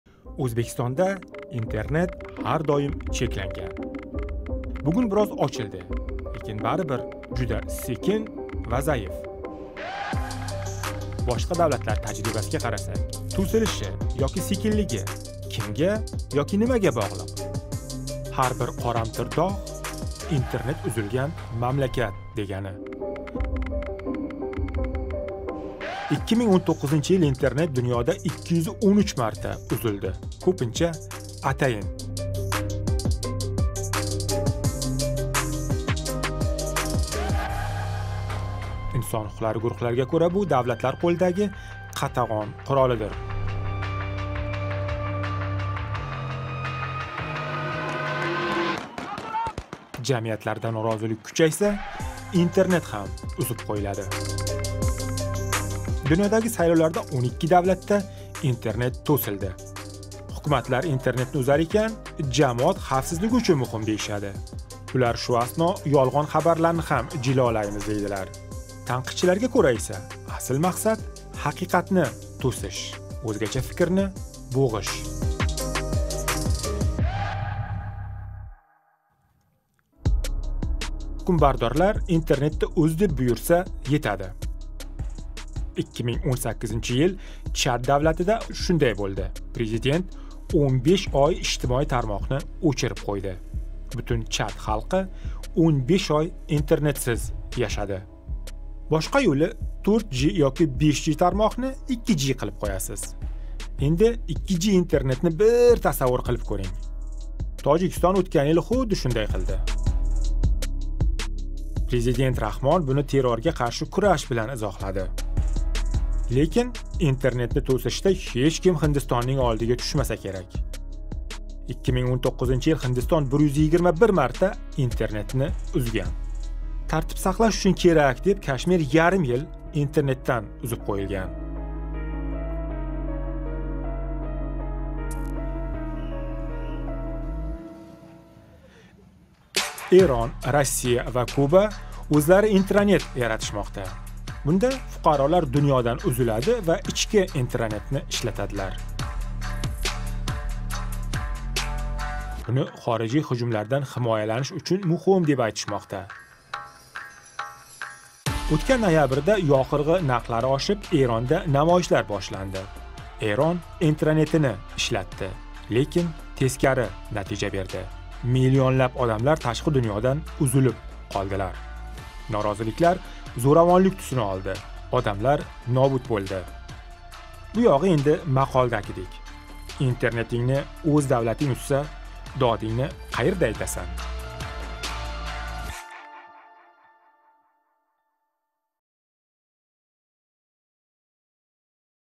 Президент уз деса бўлди, интернет узилади - Ўзбекистон ва дунё
Ўтган йил интернет 33 давлатда 200 мартадан кўпроқ узиб қўйилди. Интернетнинг ҳукумат томонидан бўғилиши дунёдаги ўнлаб миллион одамга таъсир қилди. Инсон ҳуқуқлари фаолларининг айтишича, давлат одамларга босим ўтказиш учун интернетни узиб қўямоқда.